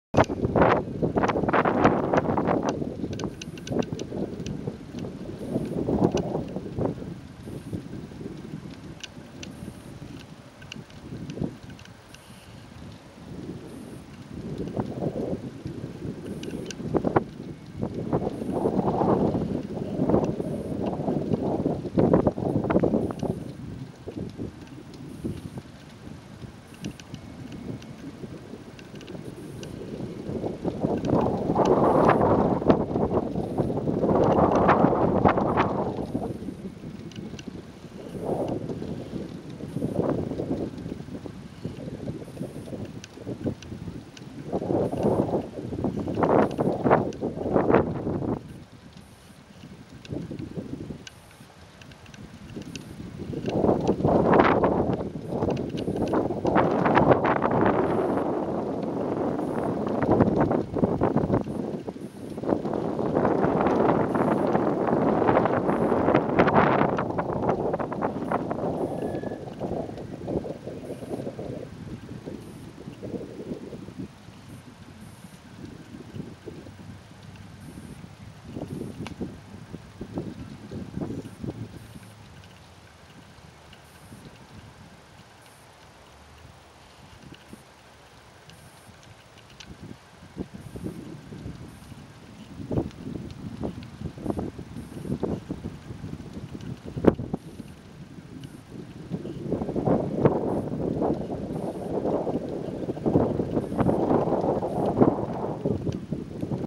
Kiteworks Diamond Kite Kit Flying
Video of me test flying one of my Paper Diamond kite Kits at Bastion point in about 5 knots